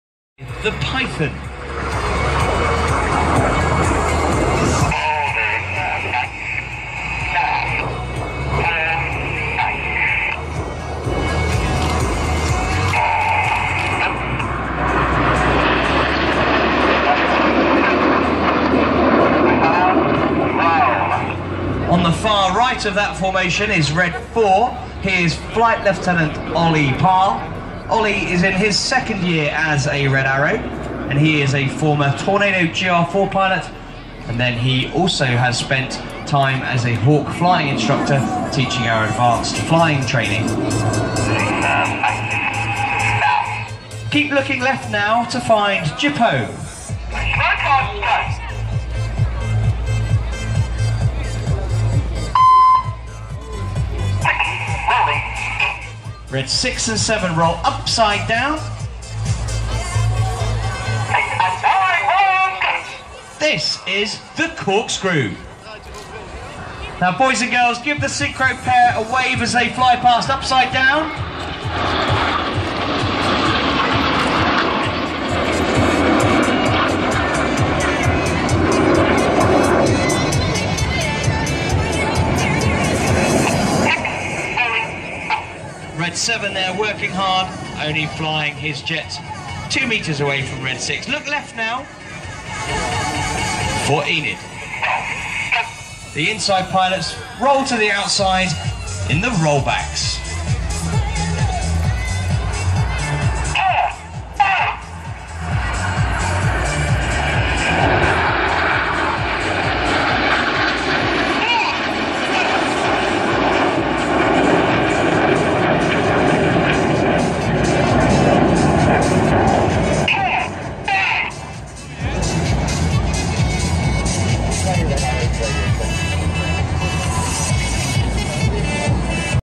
Red Arrows - 2014
RNLAF Air Force Days in the Netherlands (Gilze-Rijen) - wonderful display by the RAF Red Arrows!